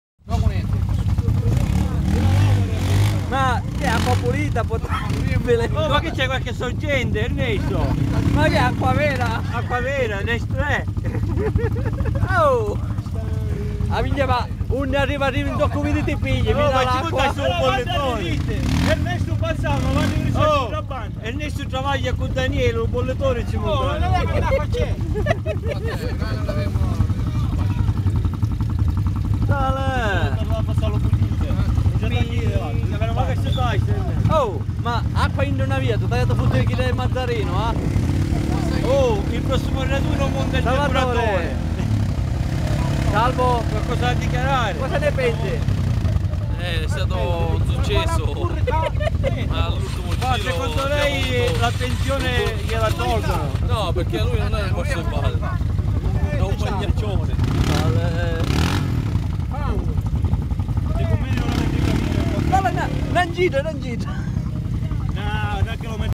1° Raduno Mazzarino
miracolooooooo,trovata sorgente di acqua potabile in un polaris scrambler di un sbiellato